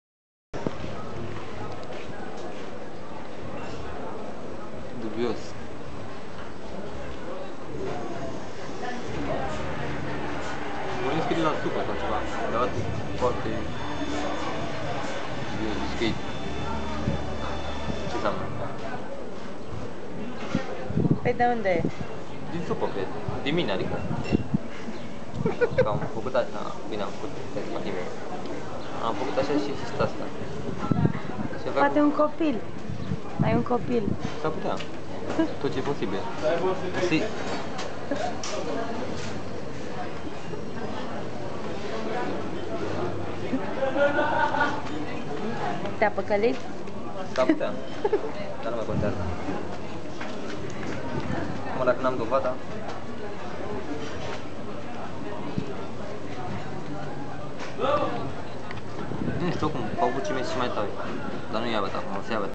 Filosofii din Vama Veche- iYli's blog
@baditaflorin a gasit noi motive de a se gandi la nemurirea vietii din Vama Veche. Desi el tot repeta ca nu stie nimic.
Tipul asta reprezinta un adevarat mister :)